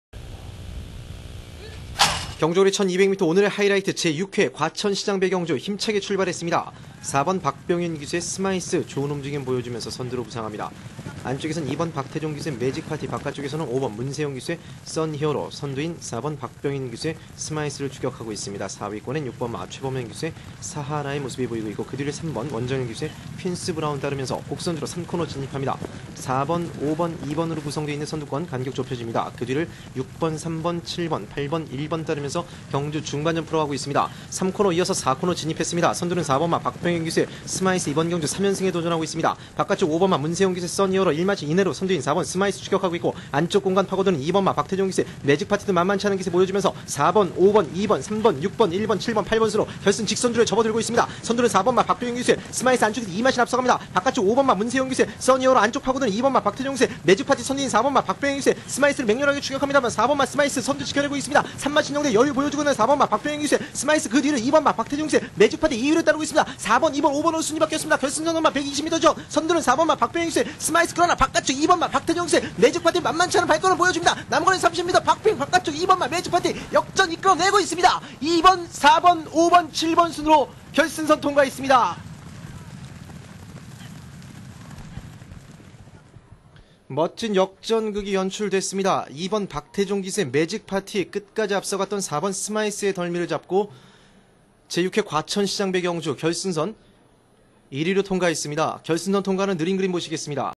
2010 Mayor of Gwacheon City Cup - Magic Party
2010 Mayor of Gwacheon City Cup - Magic Party

Seoul Racecourse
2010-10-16 Sunday, Slow, 6 furlongs.
2yo Colts & Fillies.

1) 2.Magic Party (Artie Schiller x Gigi's Magic, Allen's Prospect)
2) 4.Smyth (Consolidator x Ghostly Gal, Silver Ghost)
3) 5.Sun Hero (Menifee x Strategic Reward, Bold Revenue)

* Winning Jockey : PARK, Tae Jong

Video is from Korea Racing Authority.